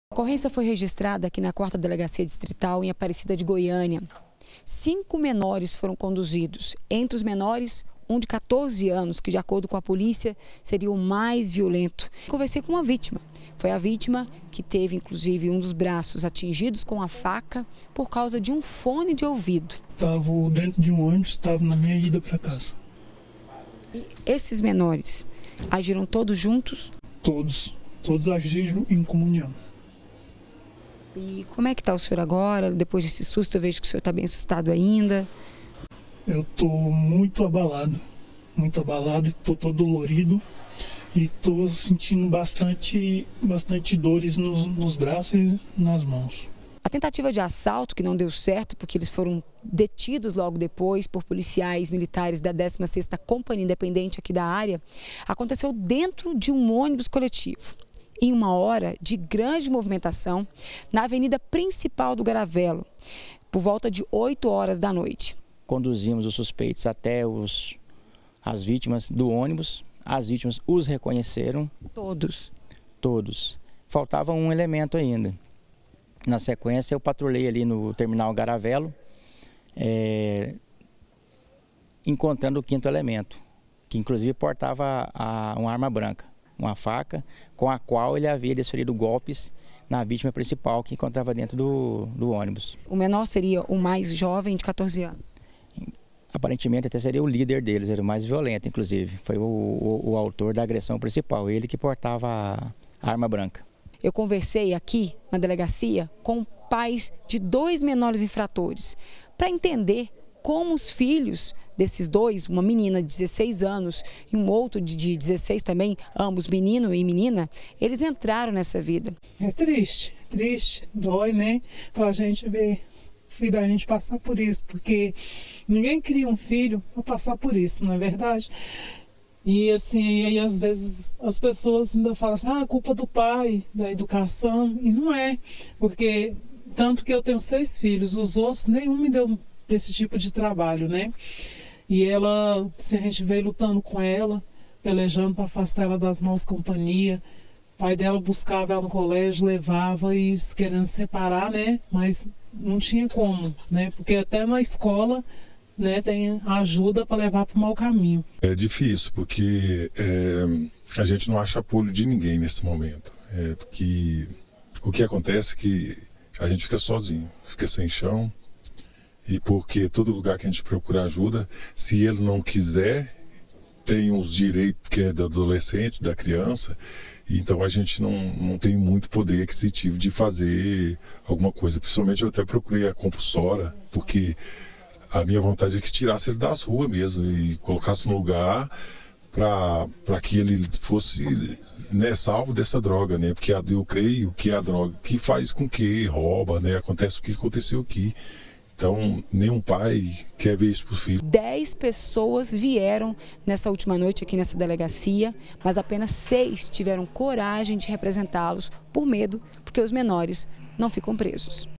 DR - Menores assaltam ônibus coletivo - 03-07-2018
Cinco menores foram apreendidos depois de terem assaltado um ônibus coletivo em Aparecida de Goiânia. Um menor de 14 anos seria o líder do grupo. Ele chegou a ferir um dos passageiros com uma faca.